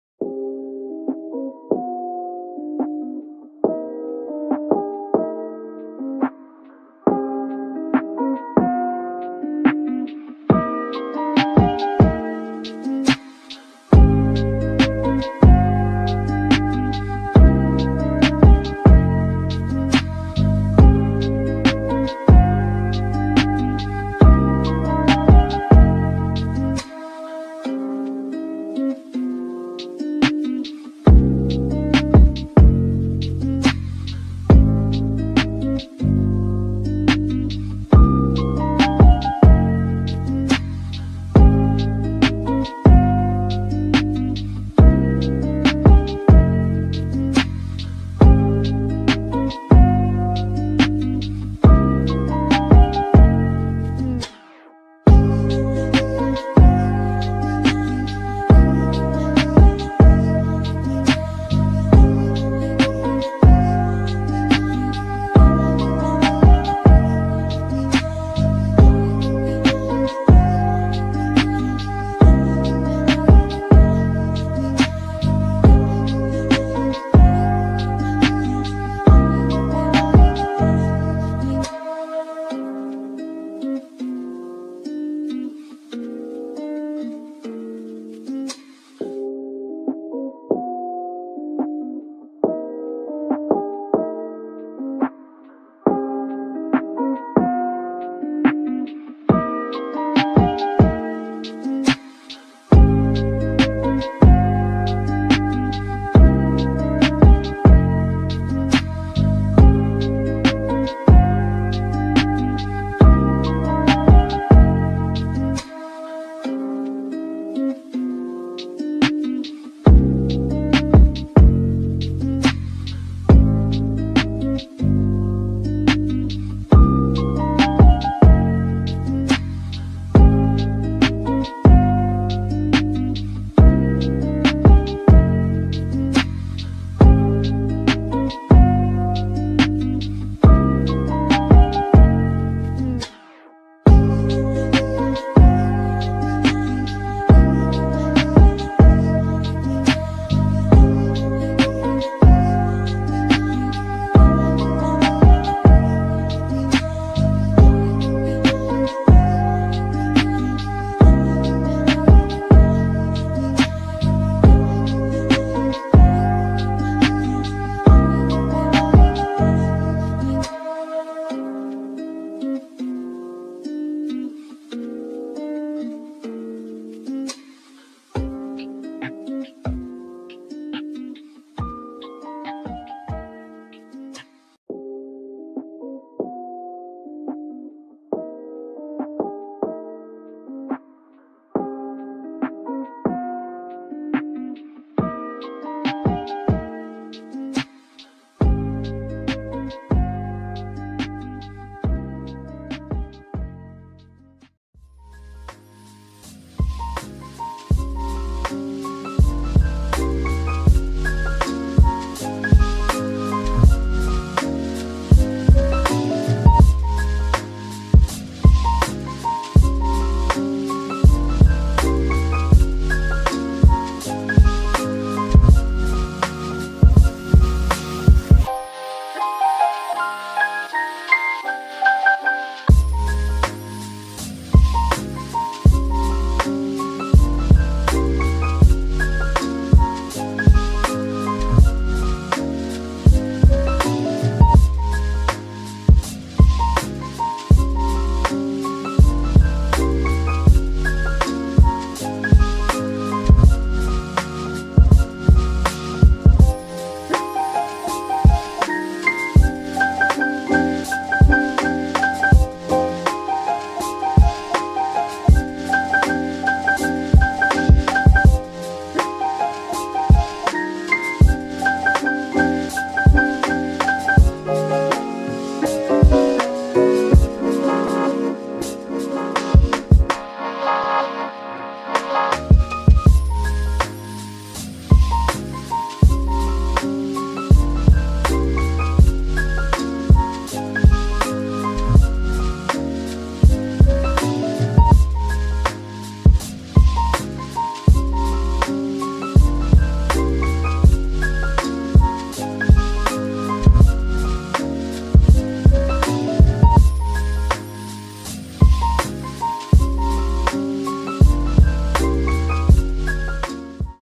Little Elephant with Balloon / Easy Acrylic Painting for Beginners
Hello! Learn how to paint a cute baby elephant with a balloon with me!